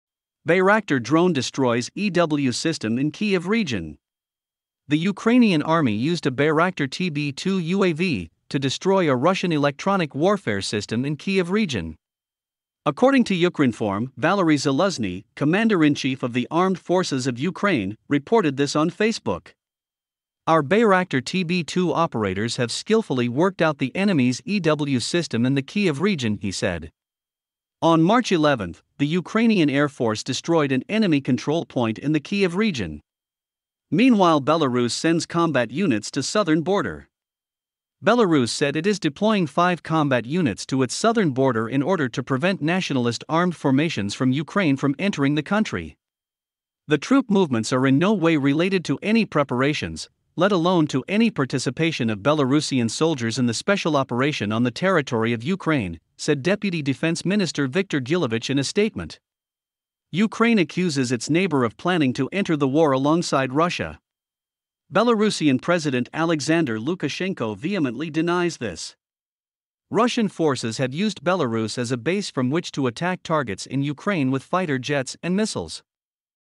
Bayraktar drone destroys EW system in Kyiv region.
The Ukrainian army used a Bayraktar TB2 UAV to destroy a Russian electronic warfare system in Kyiv region.
According to Ukrinform, Valeriy Zaluzhny, Commander-in-Chief of the Armed Forces of Ukraine, reported this on Facebook.
"Our Bayraktar TB2 operators have skillfully 'worked out' the enemy's EW system in the Kyiv region," he said.

On March 11, the Ukrainian Air Force destroyed an enemy control point in the Kyiv region.
Meanwhile Belarus sends combat units to southern border
Belarus said it is deploying five combat units to its southern border in order to prevent “nationalist armed formations” from Ukraine from entering the country.
“The troop movements are in no way related to any preparations, let alone to any participation of Belarusian soldiers in the special operation on the territory of Ukraine,” said Deputy Defence Minister Viktor Gulevich in a statement.

Ukraine accuses its neighbour of planning to enter the war alongside Russia. Belarusian President Alexander Lukashenko vehemently denies this.

Russian forces have used Belarus as a base from which to attack targets in Ukraine with fighter jets and missiles